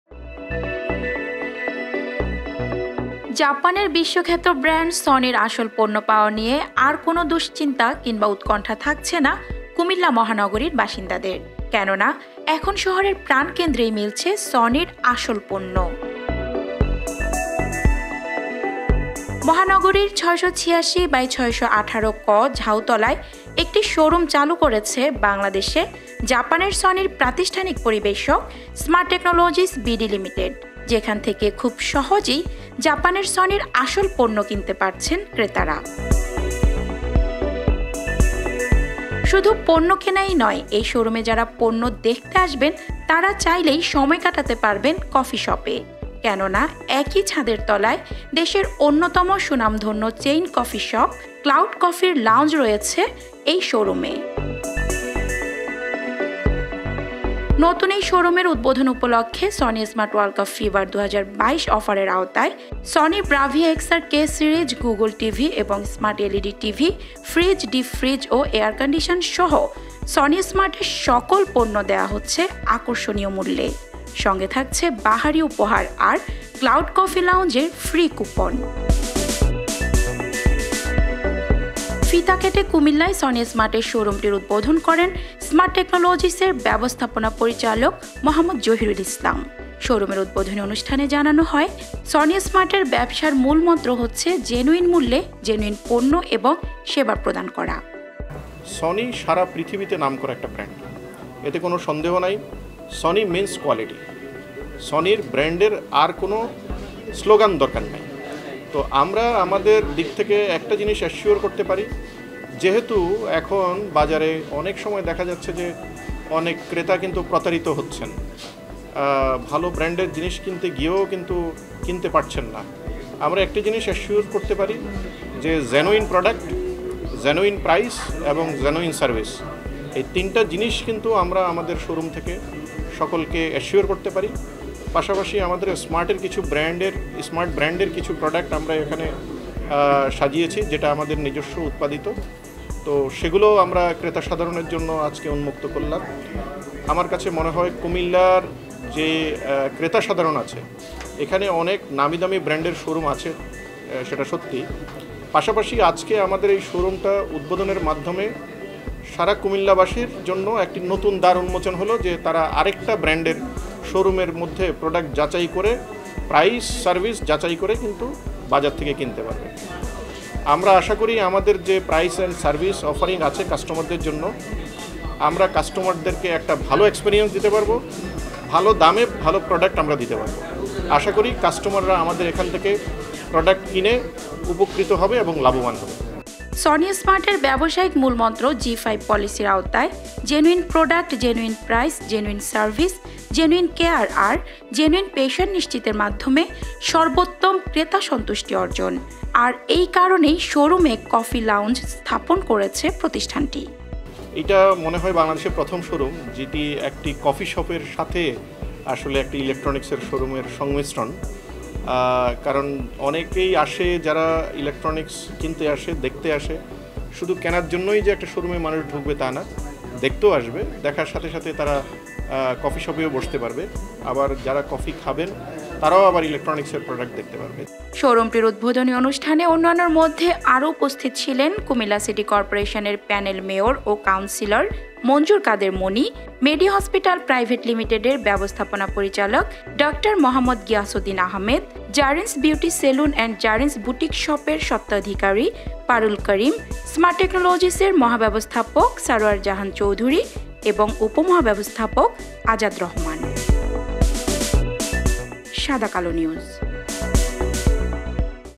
কুমিল্লায় সনির আসল পণ্য বিক্রি শুরু । Sony-Smart Cumilla Showroom । সাদাকালো নিউজ । sadakalo.news
কুমিল্লায় সনির আসল পণ্য বিক্রি শুরু । কুমিল্লায় সনি-স্মার্টের ব্যতিক্রমী শো-রুম একই ছাদের তলায় ইলেকট্রনিকস পণ্য-কফি শপ । জাপানের বিশ্বখ্যাত ব্র্যান্ড সনি’র আসল পণ্য পাওয়া নিয়ে আর কোনো দুশ্চিন্তা কিংবা উৎকণ্ঠা থাকছেনা কুমিল্লা মহানগরীর বাসিন্দাদের। কেননা এখন শহরের প্রাণকেন্দ্রেই মিলছে সনি’র আসল পণ্য।